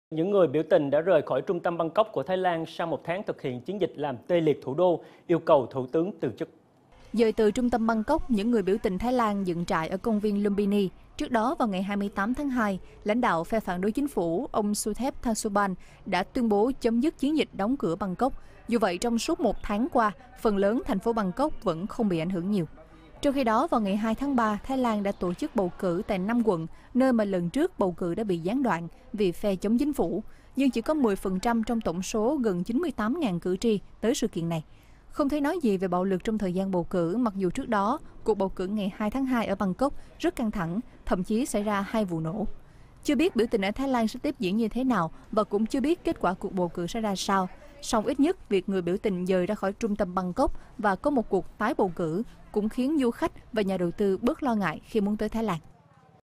Thái Lan: Người biểu tình rời khỏi Bangkok, 5 Quận tổ chức bầu cử lại
Những người biểu tình đã rời khỏi trung tâm Bangkok của Thái Lan sau một tháng thực hiện chiến dịch làm tê liệt Thủ đô, yêu cầu thủ tướng từ chức.